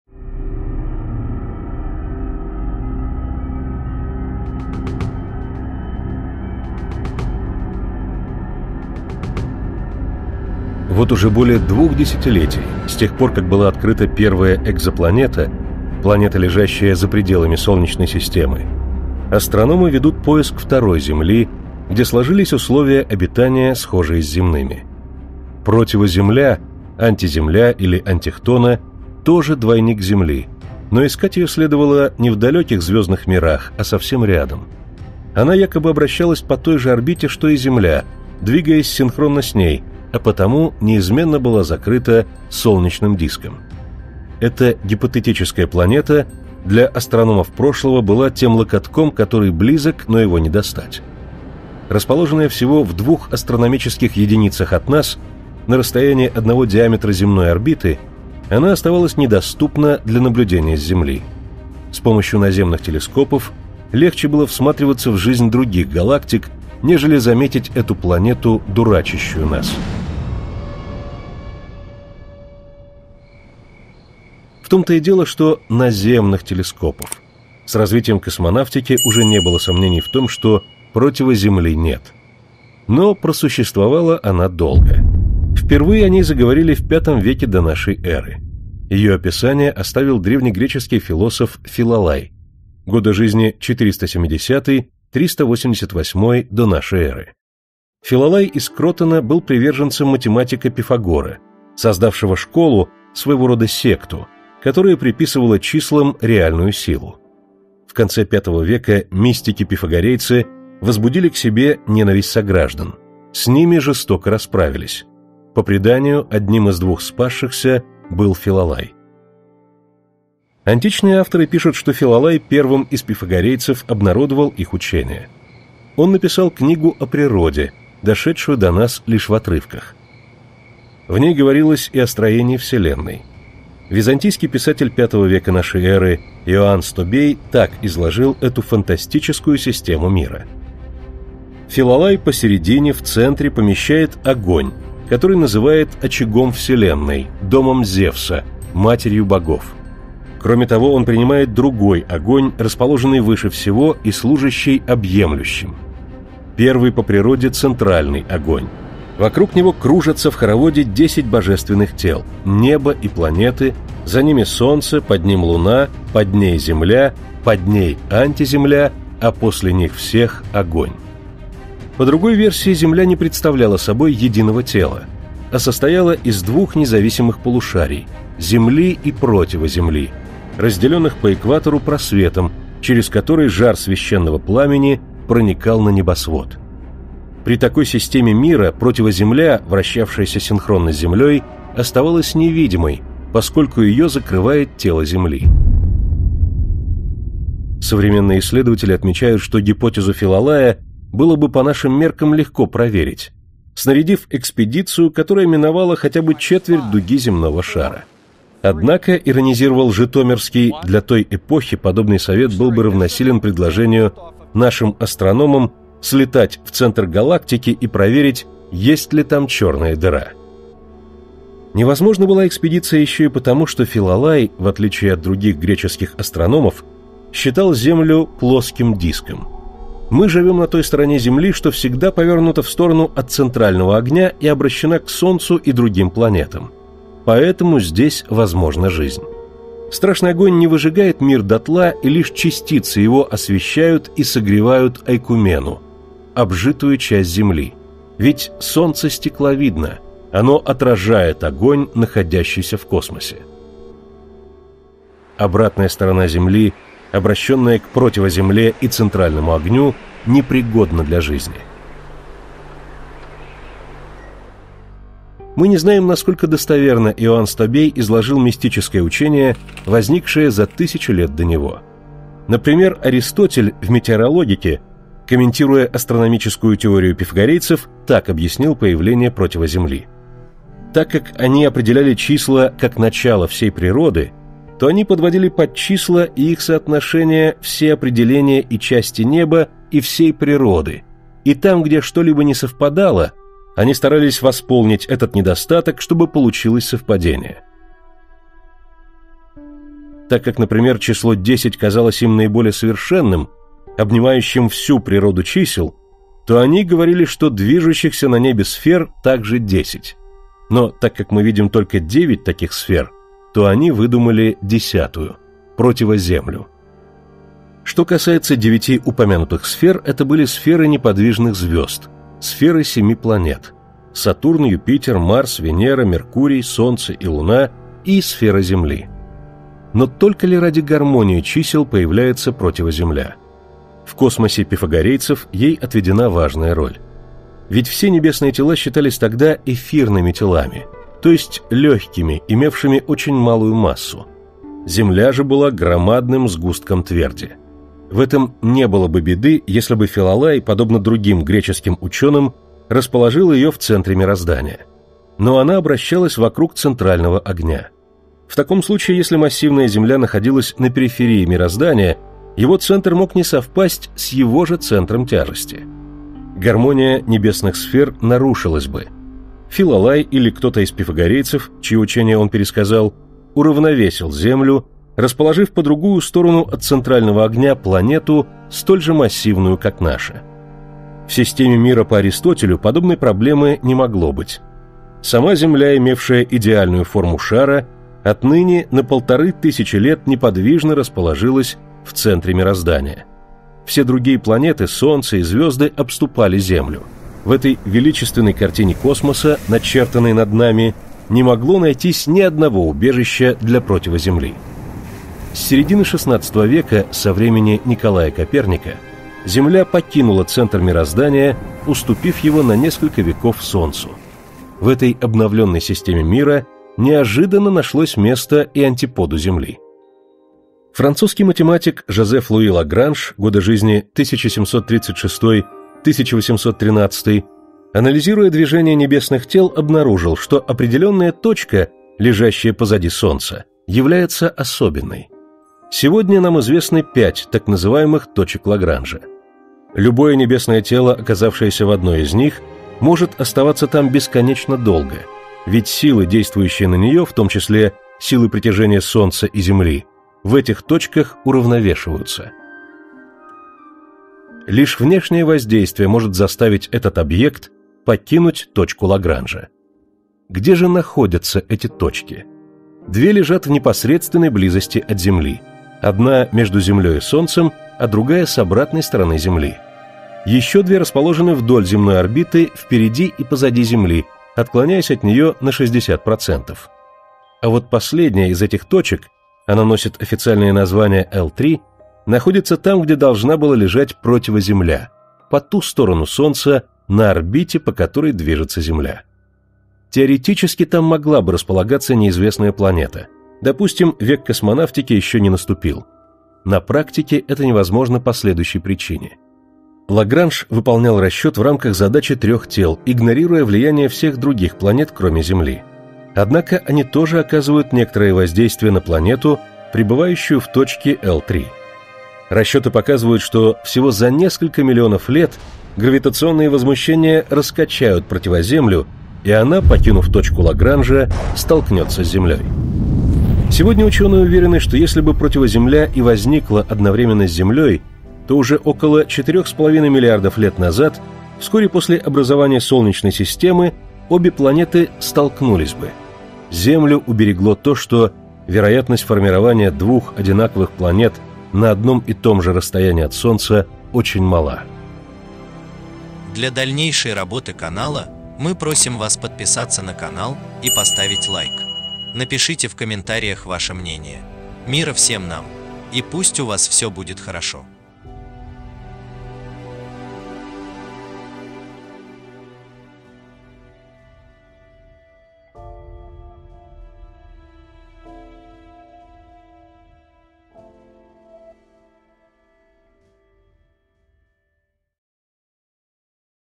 АНТИХТОН. АНТИЗЕМЛЯ. ПРОТИВОЗЕМЛЯ#история#тайна#космос#загадки#нло
Объект, находящийся на противоположной точке орбиты Земли